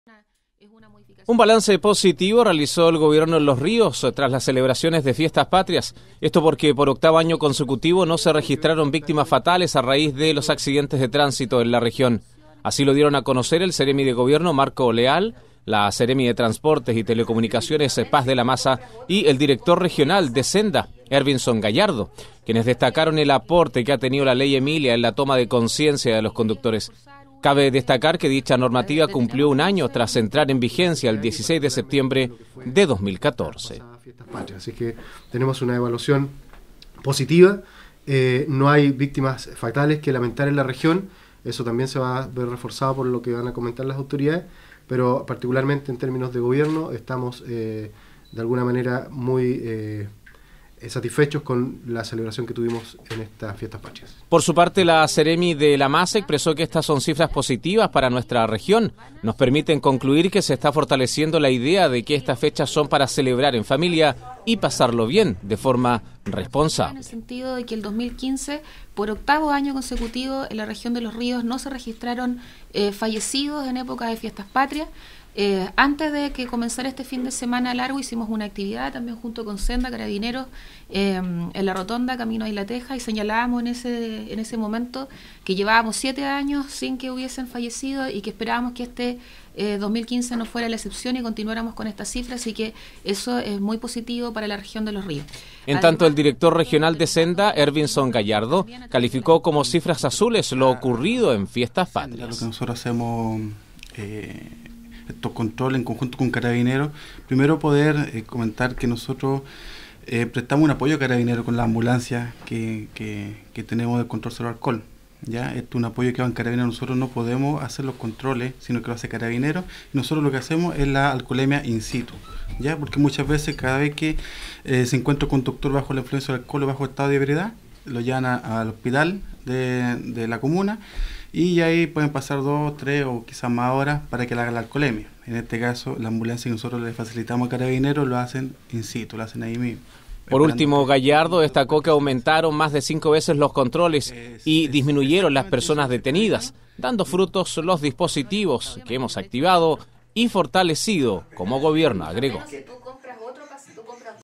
Región de Los Ríos nuevamente no tuvo fallecidos por accidentes en fiestas patrias
Gobierno destacó que, por octavo año consecutivo, no hubo fallecidos producto de accidentes de tránsito en la región de Los Ríos, recalcando el aporte que ha tenido la Ley Emilia en la toma de conciencia de los conductores.